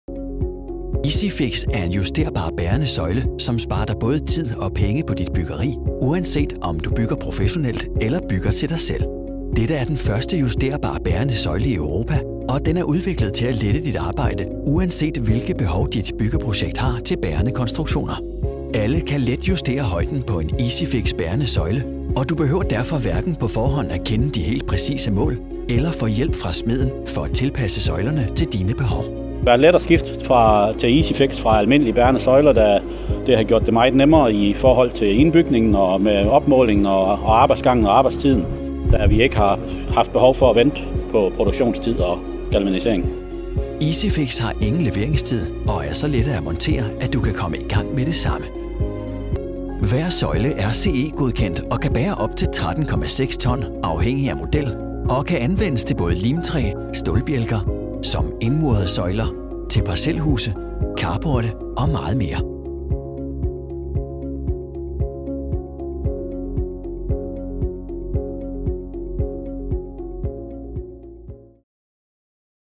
Easyfix - Spar tid på dine byggeprojekter med Easyfix
Med EasyFix bærende søjler oplever du hurtigt, at arbejdsprocessen bliver lettere. Du kan selv justere og montere søjlerne til dit projekt.